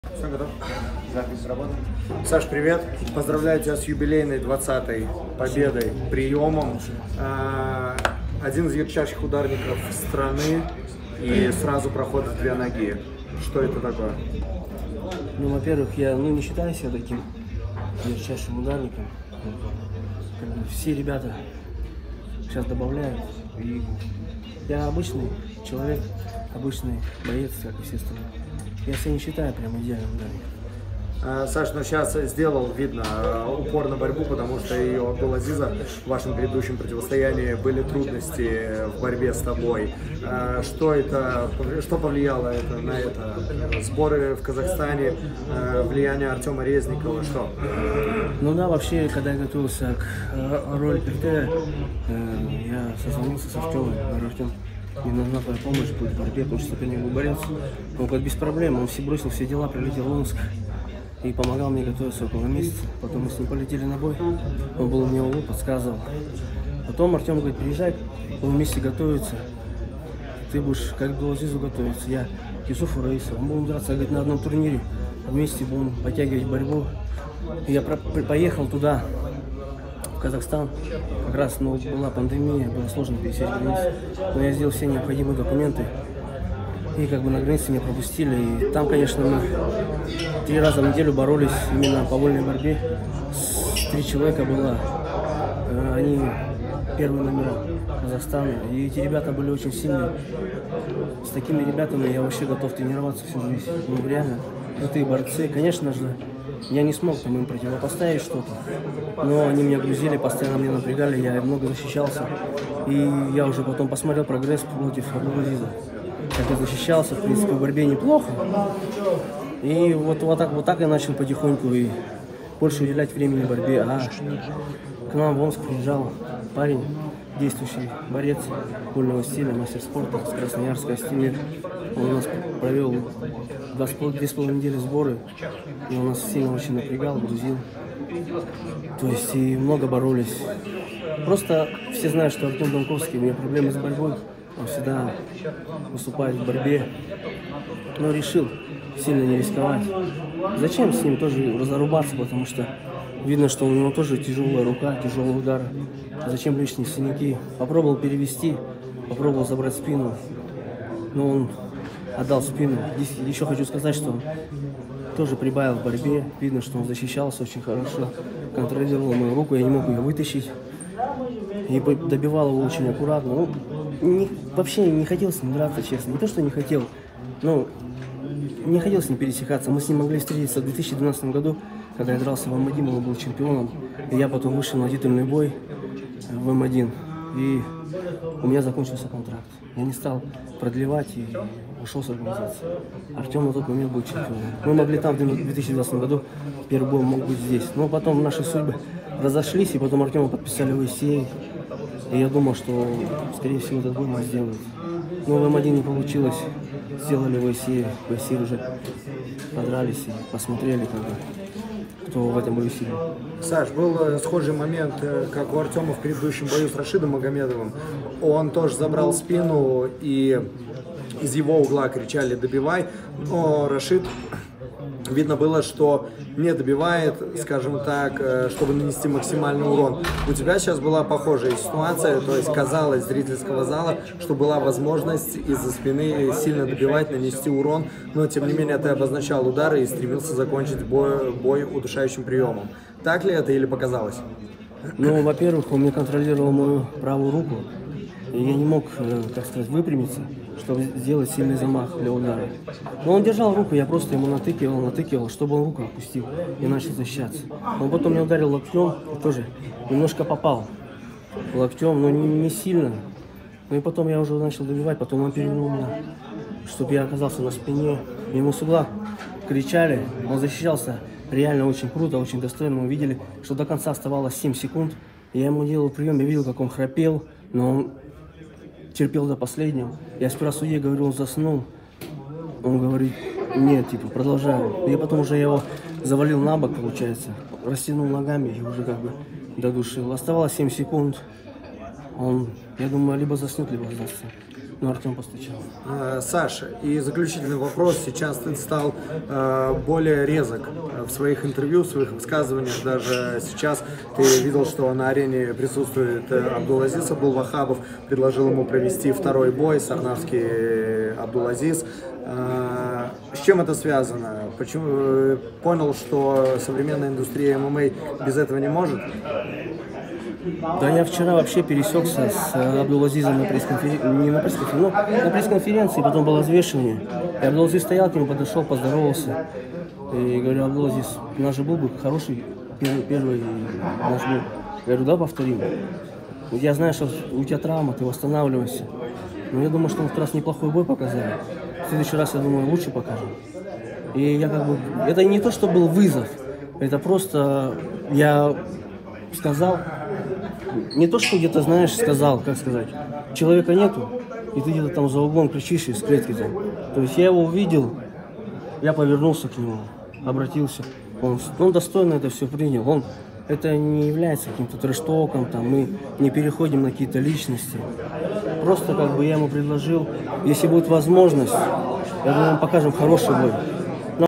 САРНАВСКИЙ БРОСИЛ ВЫЗОВ АБДУЛ-АЗИЗУ АБДУЛВАХАБОВУ / ACA 118
Александр Сарнавский поделился эмоциями после победы над Артемом Дамковским на турнире ACA 118, а также бросил вызов Абдул-Азизу Абдулвахабову.